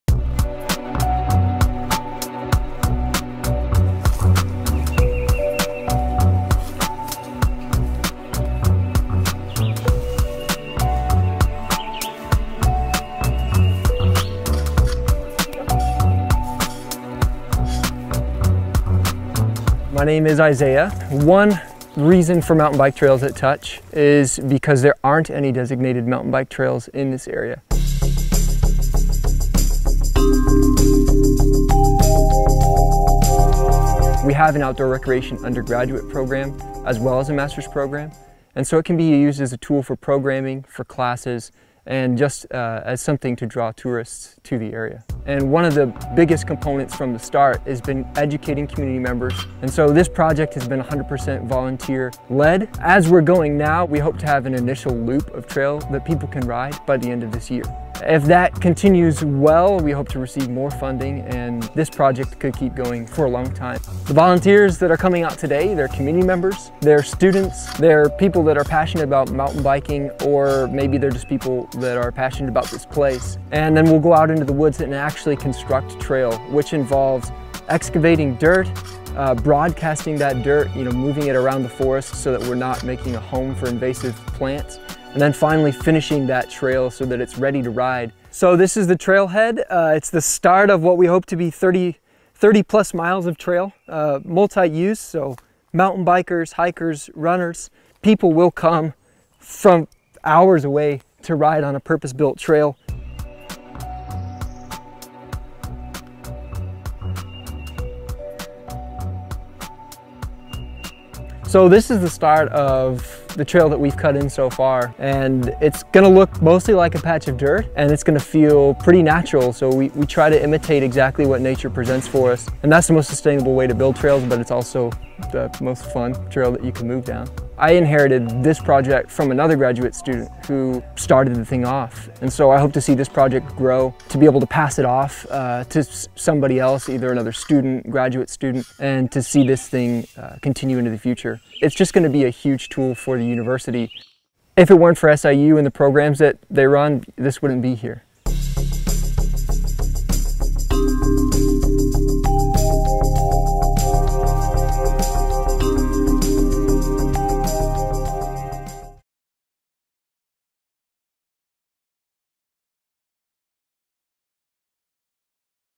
Saluki Stories: Isaiah Bike trails
Isaiah Tanner, recent graduate from SIU Carbondale's Recreation Professions Program with master's degree leading an effort to build "A Mountain Bike Trail"